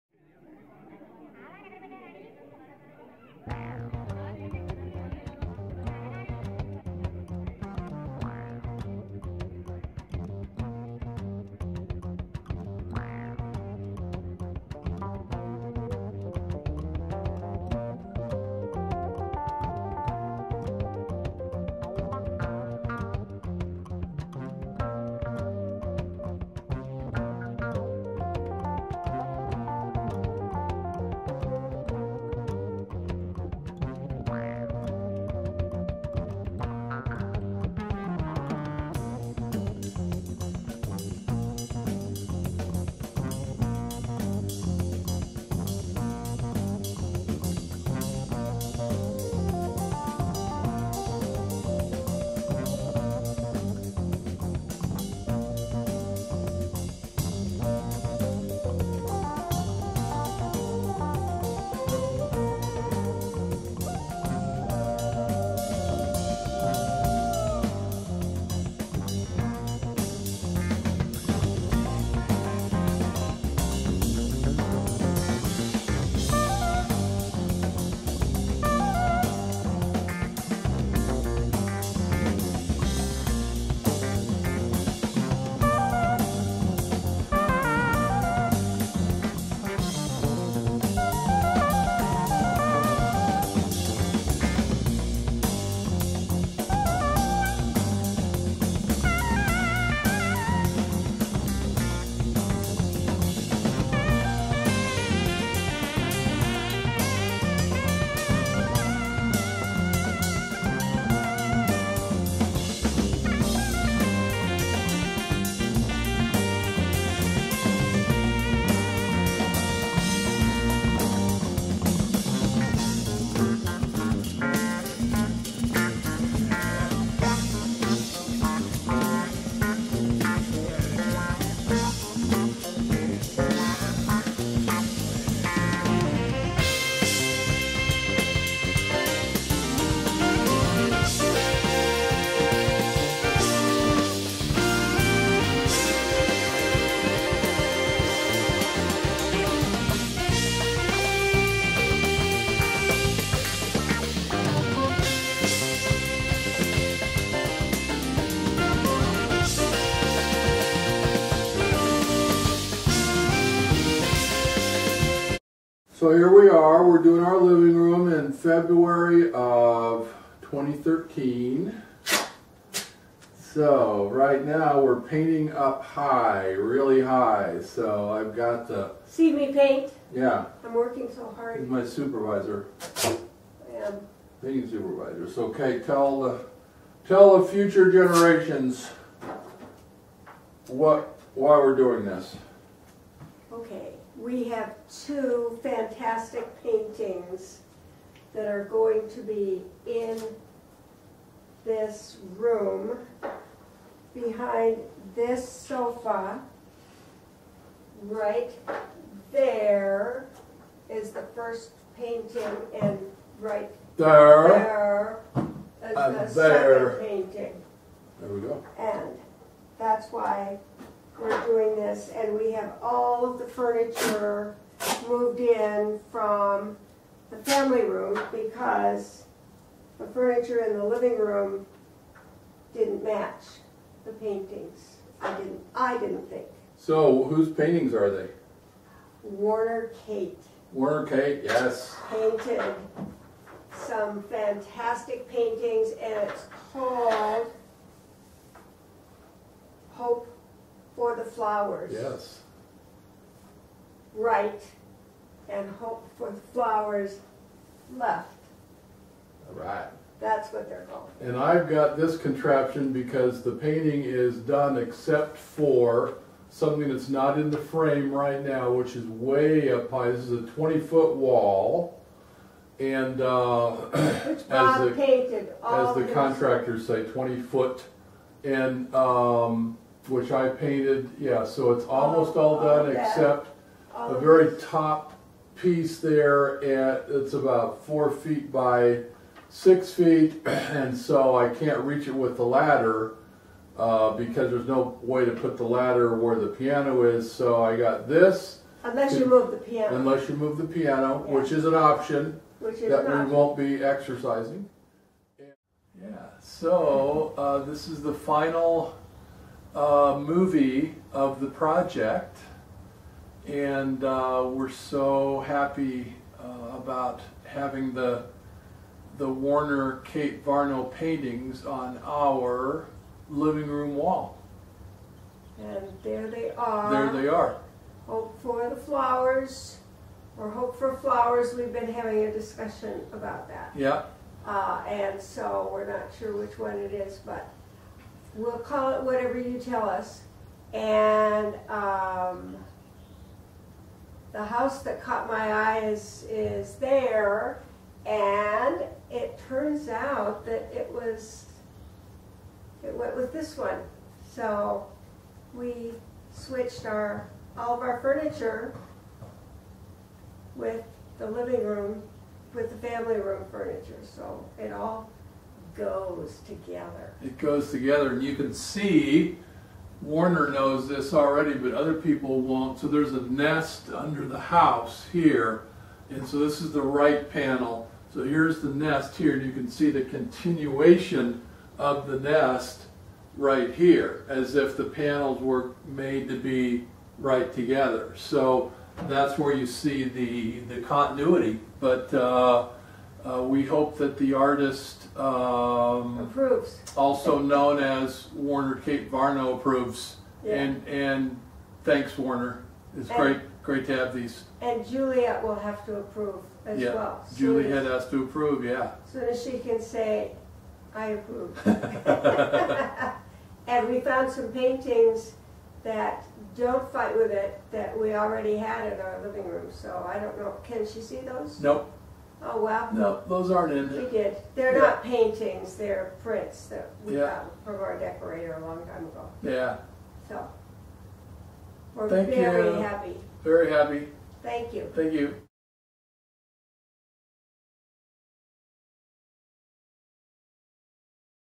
Warner Varno Paintings
Been helping Warner Varno with her web site; she asked if we'd like to hang some paintings in our house; we said "YES"!